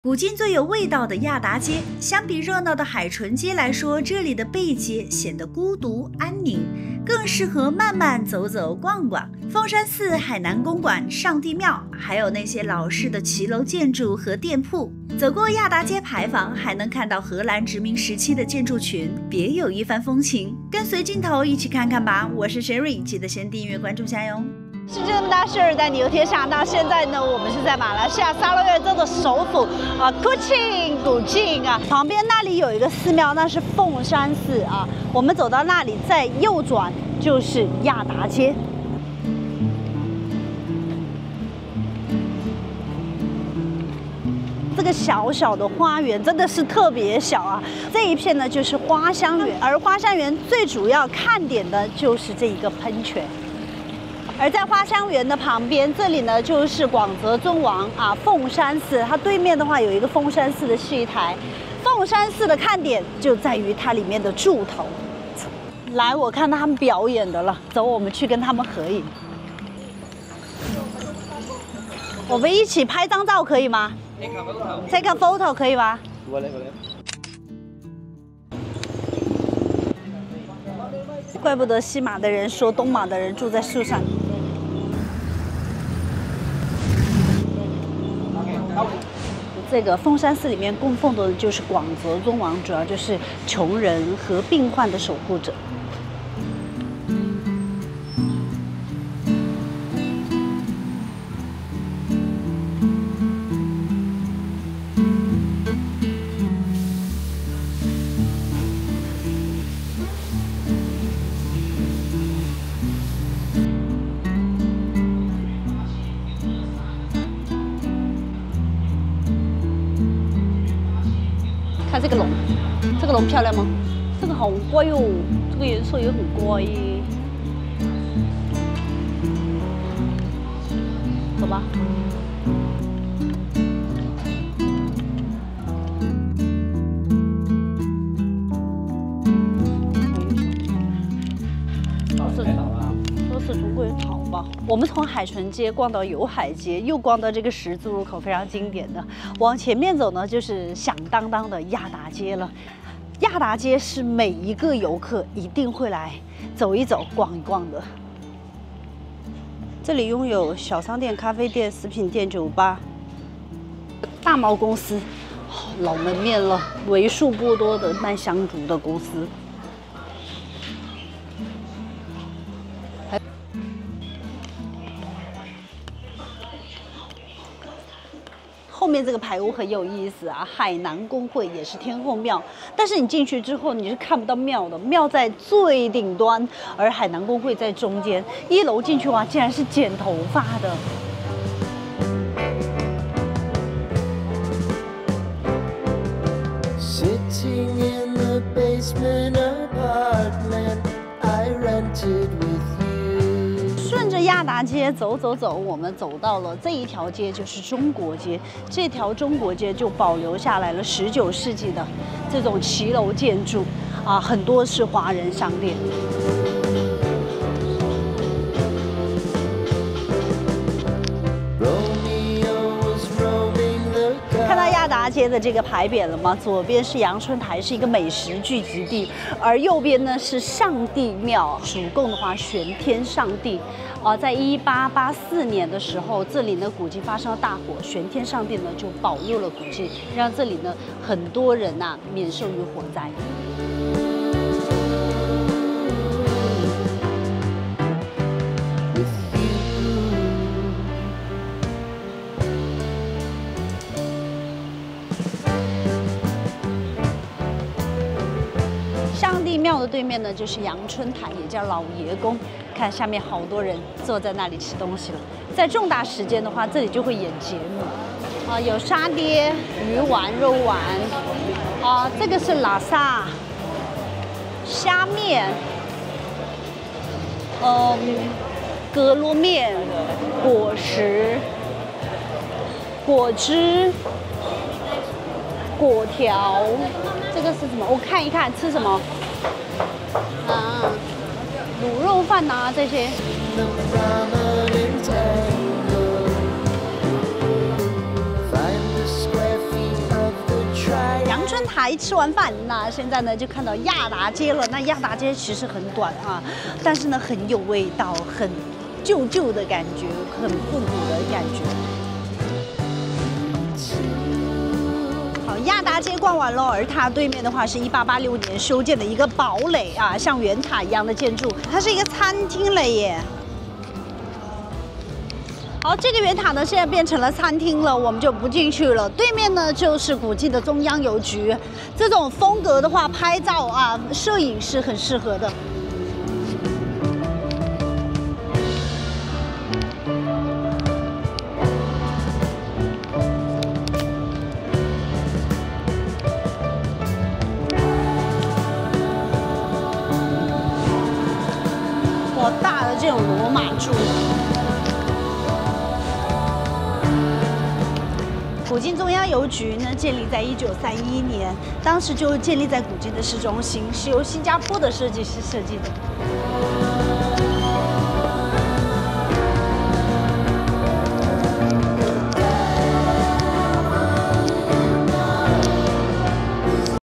马来西亚🇲🇾古晋 最有名的一条街 亚答街 从凤山寺到上帝庙 老骑楼 老商铺
古晋最有味道的亚答街｜慢游背街的宁静之美

🌿 相较于热闹的海唇街，亚答街充满了独特的静谧。这里的背街似乎更适合你慢慢走、慢慢逛，感受古晋的岁月痕迹。

🏛️ 走过这条街，你会邂逅凤山寺、海南公馆、上帝庙，以及那些古老的骑楼建筑和店铺，时间仿佛在这里静止。

🏰 穿过亚答街牌坊，还能见到荷兰殖民时期的建筑群，别具一格的建筑风貌让人不禁沉醉其中。

📸 跟随镜头，和我一起探索这条古老的街道吧！